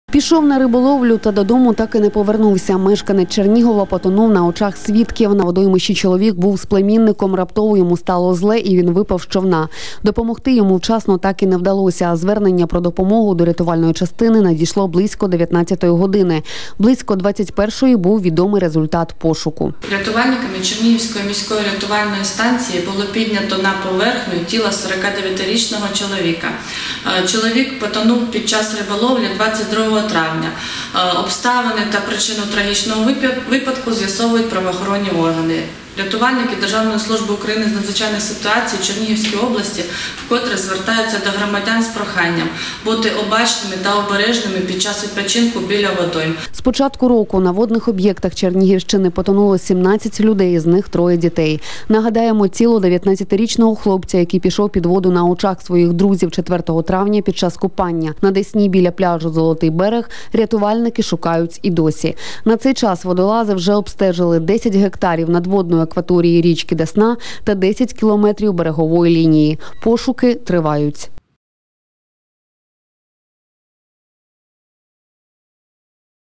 Фатальна рибалка|Телеканал Новий Чернігів
Чернігівець потонув на очах у свідків.